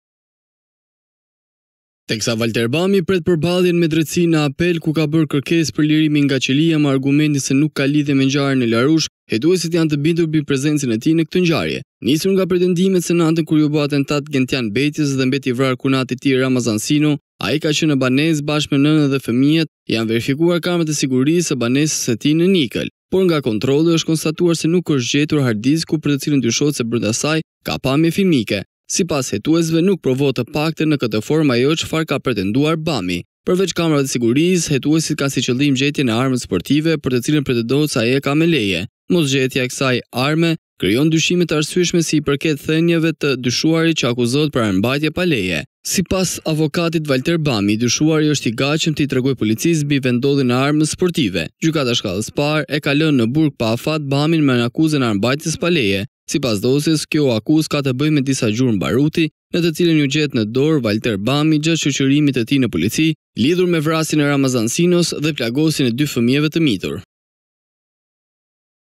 Hetimet ndaj Bamit, policia nuk gjen hard diskun e kamerave të sigurisë në shtëpi
Teksa Valter Bami pret përballjen me drejtësine në apel ku ka bërë kërkesë për lirim nga qelia me argumetin se nuk ka lidhje me ngjarjne e ndodhur në Larushk hetuesit janë të bindur mbi prezencen e tij në ngjarje.

Nisur edhe nga pretendimet se natën kur iu bë atentat Gentian Bejtjes dhe mbeti i vrarë kunati i tij Ramazan Sino ai ka qënë në banesë bashkë me nënë dhe fëmijët janë verifikuar kamerat e sigurisë së banesës në Nikël.

Por nga kontrolli është konstatur se nuk është gjetur hard disku për të cilën dyshohet se brenda saj ka pamje filmike. Sipas hetuesve, nuk provohet të paktën në këtë formë ajo çfarë ka pretenduar Bami.

Përveç kamerave hetusit kanë si qëllim gjetjen e amrës sportive për të cilën pretendohet se  ai  e ka me leje.

Mosgjetja e saj, krijon dyshime të arsyeshme sa i përket thënieve të të dyshuarit që akuzohet për armëmbajtje pa leje. Sipas avokatit të Valter Bamit, i dyshuari është i gatmshëm ti tregojë policisë mbi vendodhëjn e armës sportive.

Gjykata e shkallës së parë e ak lënë në burg pa afat Bamin me akuzën e armenmbajtjes pa leje.

Sipas dosjes kjo u akuzë ka të ëbëjë më disa gjurmë baruti në të cilën iu gjet në dorë Valter Bamit gjatë shoqërimit të tij në polici lidhur me vrasjën e Ramazan Sinos dhe plagojsjëne dy fëmije të mitur.